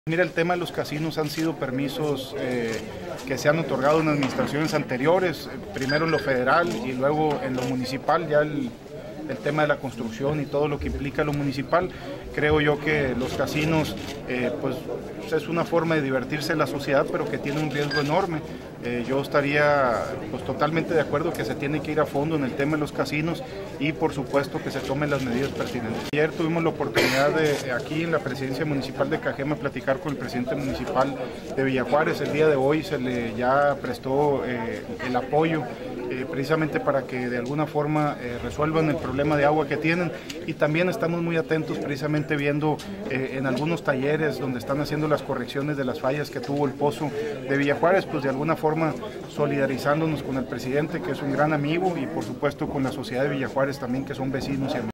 De acuerdo Alcalde de Cajeme con controlar instalación de casinos
Medidas para controlar la instalación de casinos en el estado. El Alcalde Rogelio Díaz Brown Ramsburgh destacó estar de acuerdo con que se controle la instalación y operación de casinos.